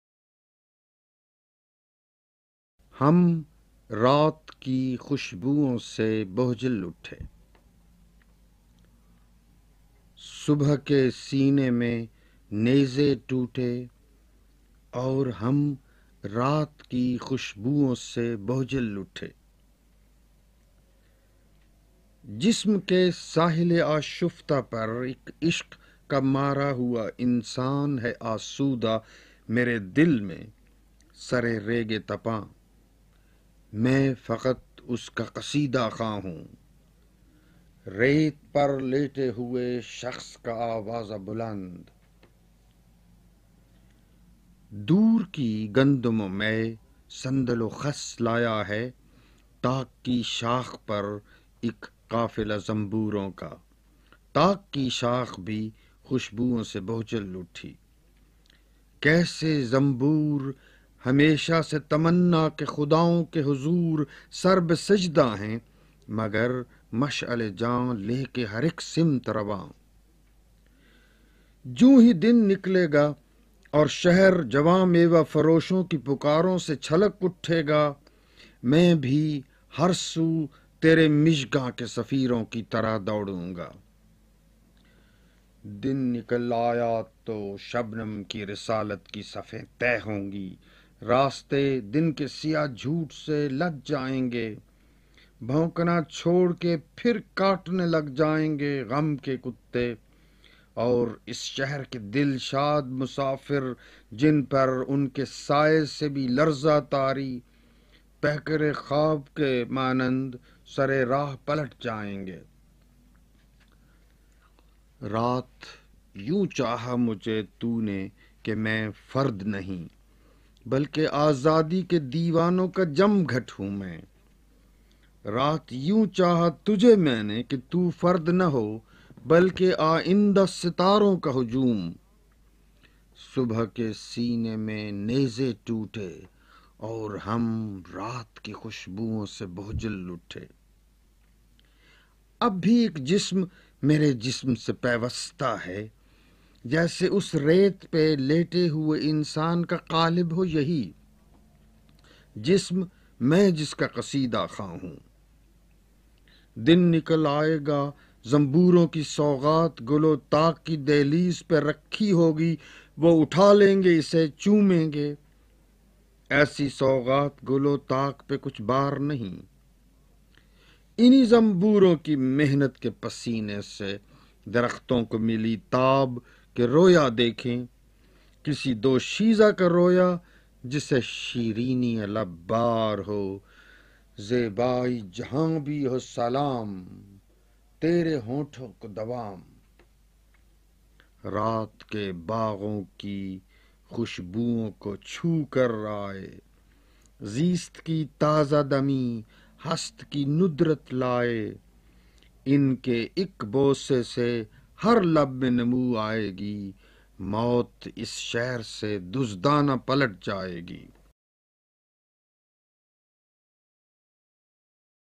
Noon Meem Rashid recites his nazm (13) - Exclusive Recording for Audio Archives of Lutfullah
ن م راشد
"ہم رات کی خوشبوؤں سے بوجھل اُٹھے "
صبح کے سینے میں نیزے ٹُوٹے ،
اور ہم رات کی خوشبوؤں سے بوجھل اُٹھے !

جسم کے ساحلِ آشفتہ پر اک عشق کا مارا ہُوا
انسان ہے آسودہ، مرے دل میں ، سرِ ریگِ تپاں
میں فقط اُس کا قصیدہ خواں ہوں !
(ریت پر لیٹے ہوئے شخص کا آوازہ بلند!)
دوُر کی گندم و مے ، صندل و خس لایا ہے
تاک کی شاخ پر اِک قافلہ زنبوروں کا!
تاک کی شاخ بھی خوشبوؤں سے بوجھل اُٹھی!
کیسے زنبور ہمیشہ سے تمنّا کے خداؤں کے حضور
سر بسجدہ ہیں ، مگر مشعلِ جاں لے کے ہر اِک سمت رواں !

جونہی دن نکلے گا اور شہر
جواں میوہ فروشوں کی پُکاروں سے چھلک اُٹھے گا،
میں بھی ہر سُو ترے مژگاں کے سفیروں کی طرح دوڑوں گا!
(دن نکل آیا تو شبنم کی رسالت کی صفیں تہہ ہوں گی
راستے دن کے سیہ جھوٹ سے لد جائیں گے
بھونکنا چھوڑ کے پھر کاٹنے لگ جائیں گے غم کے کتّے
اور ِاس شہر کے دلشاد مسافر، جن پر
اُن کے سائے سے بھی لرزہ طاری،
پیکرِ خواب کے مانند سرِ راہ پلٹ جائیں گے )
رات یوں چاہا مجھے تُو نے کہ مَیں فرد نہیں
بلکہ آزادی کے دیوانوں کا جمگھٹ ہُوں میں ؛
رات یوں چاہا تجھے مَیں نے کہ تُو فرد نہ ہو
بلکہ آئندہ ستاروں کا ہجوم۔ ۔ ۔
اور ہم رات کی خوشبوؤں سے بوجھل اٹھے !

اب بھی اِک جسم مرے جسم سے پیوستہ ہے
جیسے اُس ریت پہ لیٹے ہوئے انسان کا قالب ہو یہی ۔۔
جسم، مَیں جس کا قصیدہ خواں ہُوں ۔۔۔
دن نکل آئے گا زنبوروں کی سوغات گُل و تاک
کی دہلیز پہ رکھی ہو گی،
وہ اُٹھا لیں گے اسے چومیں گے
ایسی سوغات گُل و تاک پہ کچھ بار نہیں!
انہی زنبوروں کی محنت کے پسینے سے درختوں کو مِلی
تاب، کہ رؤیا دیکھیں
کسی دوشیزہ کا رؤیا جسے شیرینیِ لب بار ہو
(زیبائی جہاں بھی ہو سلام ۔۔۔
تیرے ہونٹوں کو دوام!)
رات کے باغوں کی خوشبوؤں کو چُھو کر آئے،
زیست کی تازہ دمی، ہست کی نُدرت لائے،
اِن کے اِک بوسے سے ہر لب میں نمو آئے گی
موت اِس شہر سے دزدانہ پلٹ جائے گی

ن م راشد